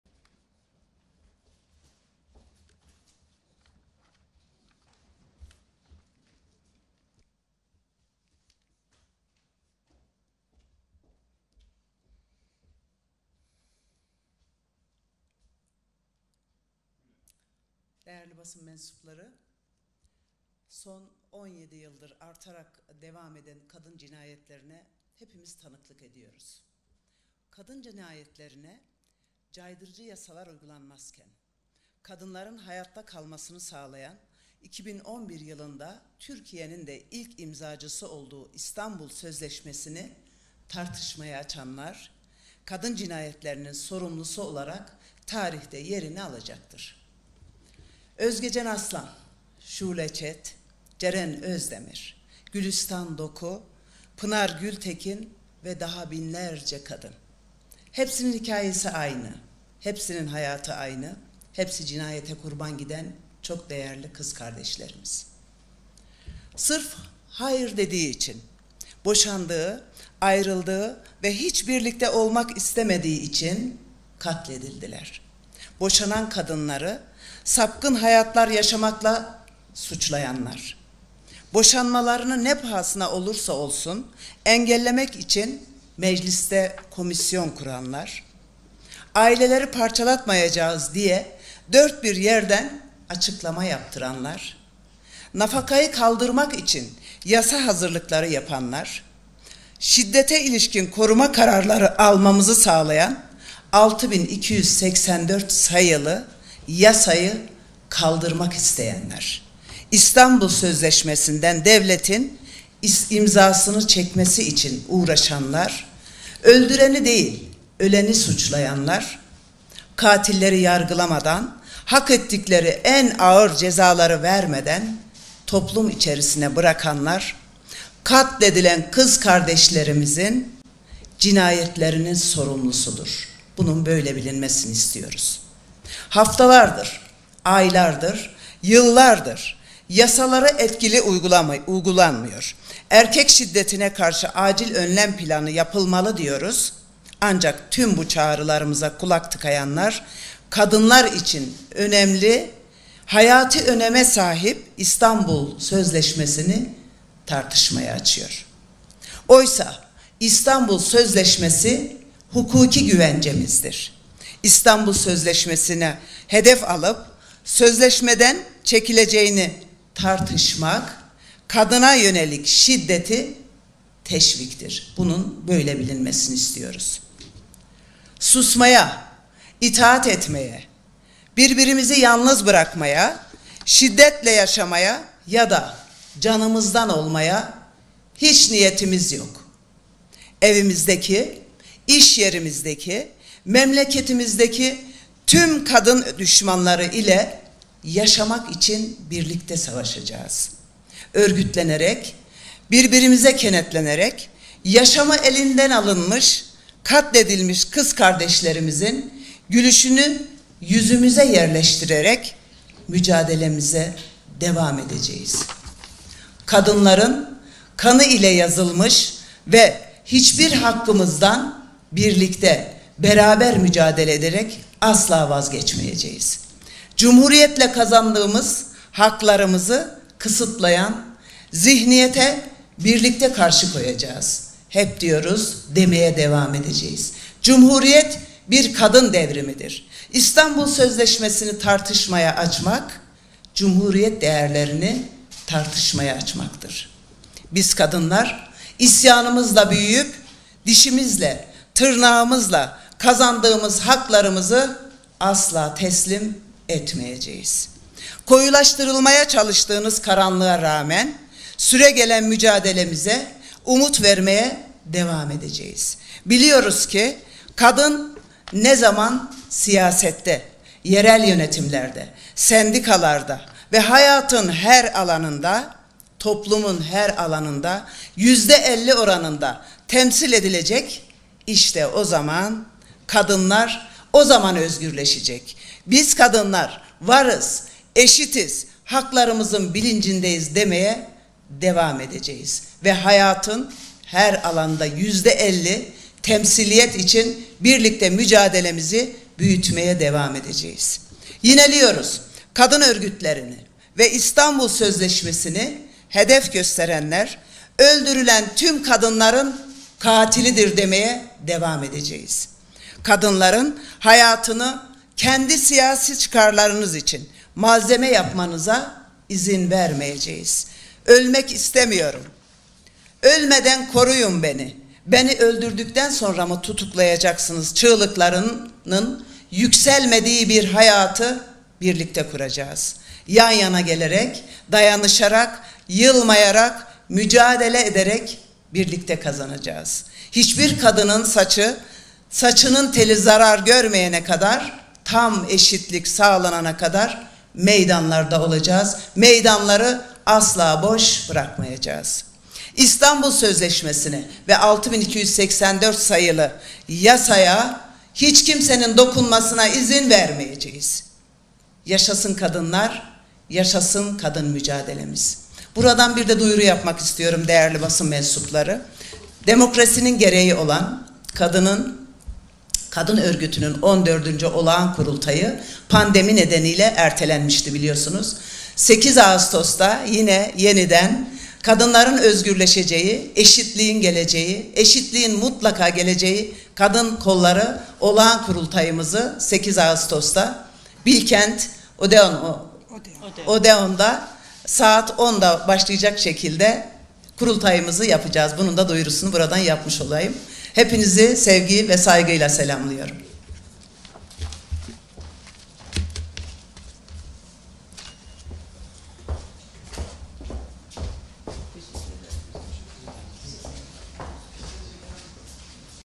FATMA KÖSE BASIN TOPLANTISI 28/07/2020
Cumhuriyet Halk Partisi Kadın Kolları Başkanı Fatma Köse basın toplantısı düzenledi.